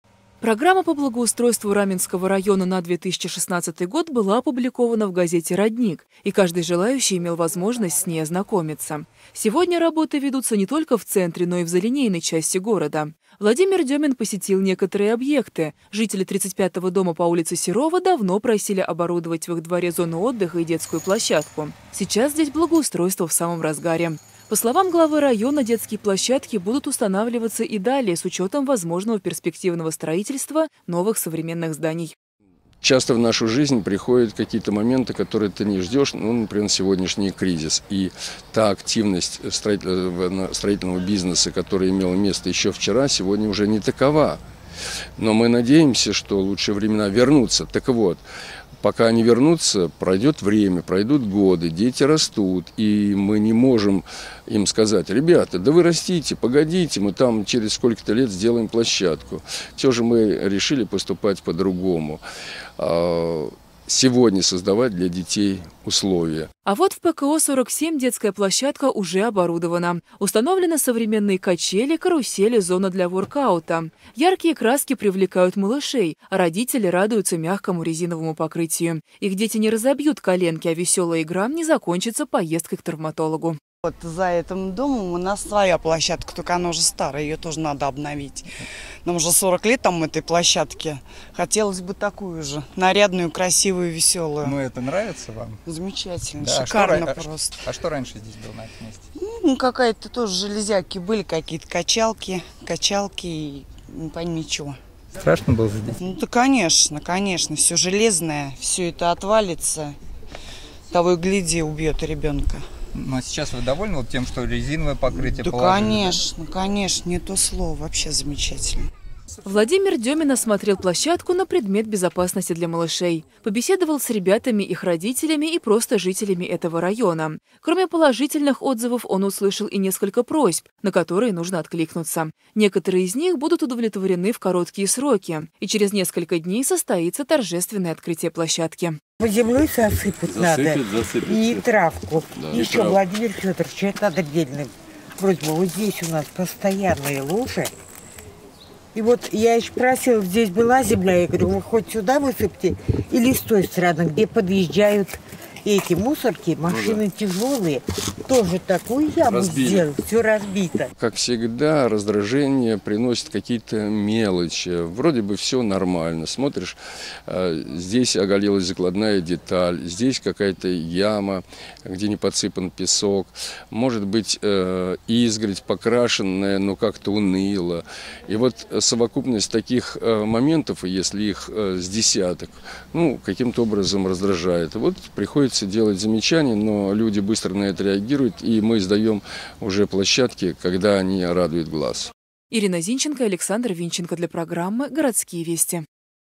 25 3
Наш город с каждым годом становится все краше: появляются новые скверы и объекты благоустройства. Сейчас ведутся работы в залинейной части города.